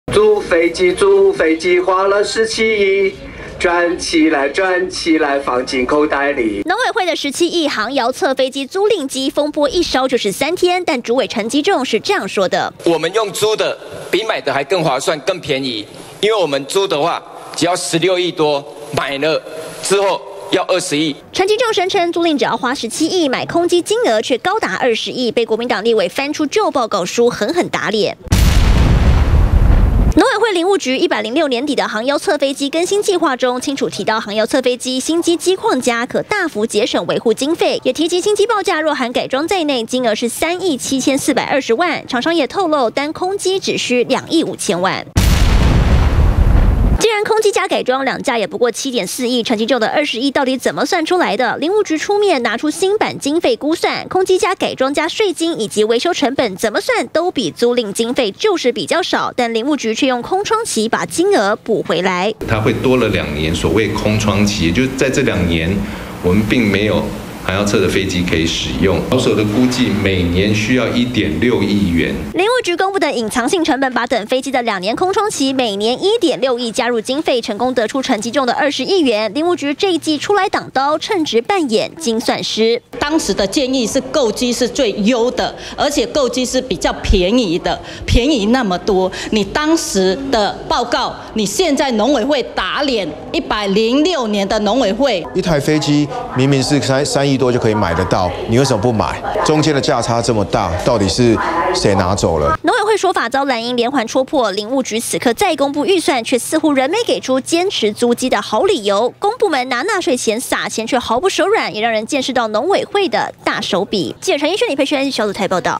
20191016中天新聞 租比買貴農委會自打臉！ 106年報告「應購新機」
農委會航遙測飛機風波，現在再被翻出106年底，林務局自己的計畫書中，明明建議購買新機，可大幅節省維護經費，如今主委陳吉仲卻對外聲稱，不僅買不如用租的，甚至還喊出20億的報價，林務局也趕緊公布新經費估算表，納入等飛機「空窗期」，硬是把陳吉仲的20億扣打給補滿。